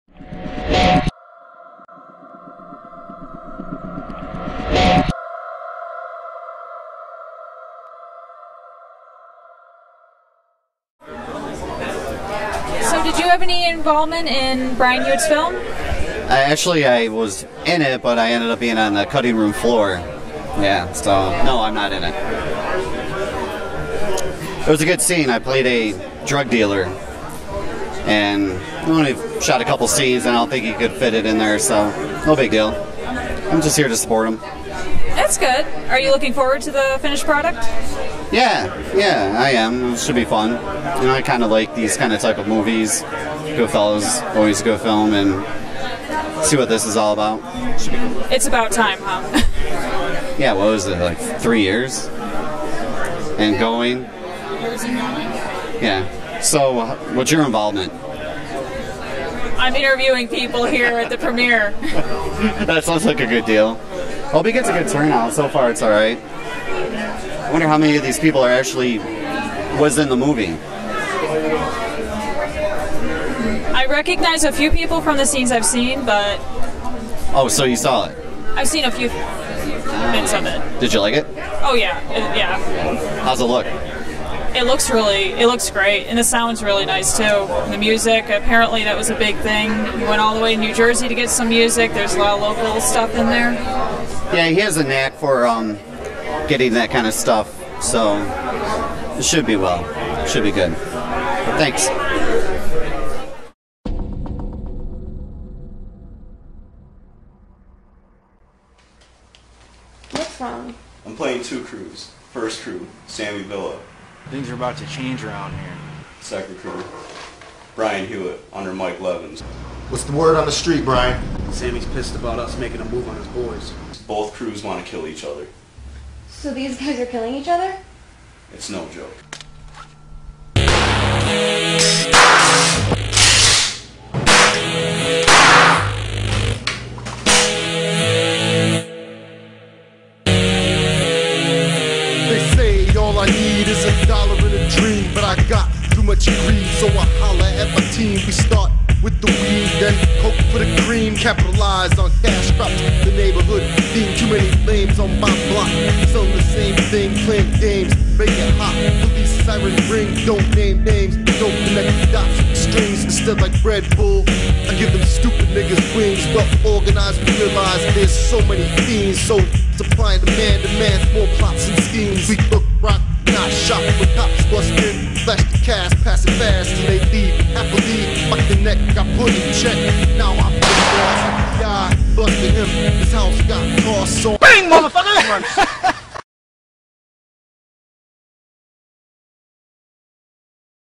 The Fiends Episode 187: Jeff Meyer
Madeline Milden interviews actor and film promoter Jeff Meyer at the premiere of the Brian Hewitt film RISE UP AND FALL, at the The Palace Theatre in Syracuse, New York on Thursday June 28th 2012.